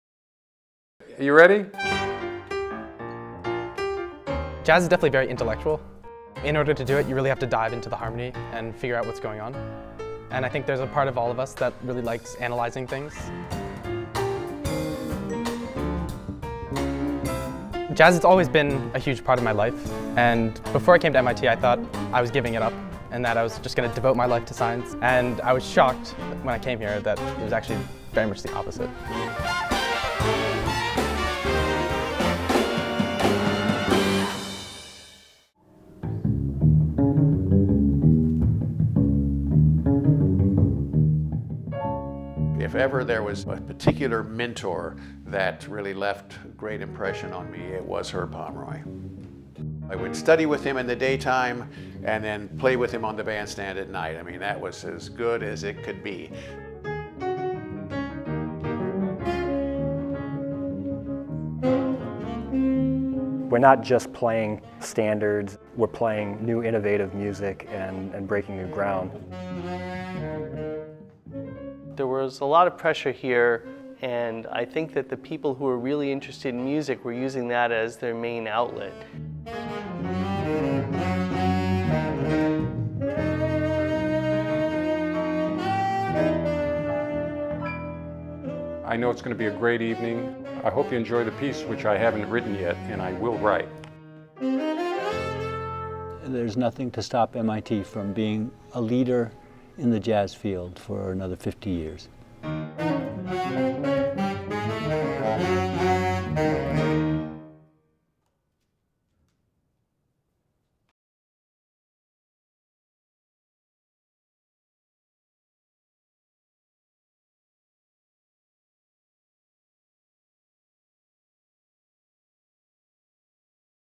MIT JAZZ 50th
Opening of the Herb Pomeroy/MIT Jazz@50 Exhibit
3-5pm

Friday, April 26
Forums: Jazz at MIT: Excellence, Spirit, and Community
Herb Pomeroy's Boston: Big Bands, Berklee, and the Bridge to MIT
4-6:30pm

Saturday, April 27
50th Anniversary Gala Concert/6th Annual Herb Pomeroy Memorial Concert
7:30pm
Featuring
MIT Festival Jazz Ensemble, MIT Alumni Jazz Band
Guest Pianist Steve Kuhn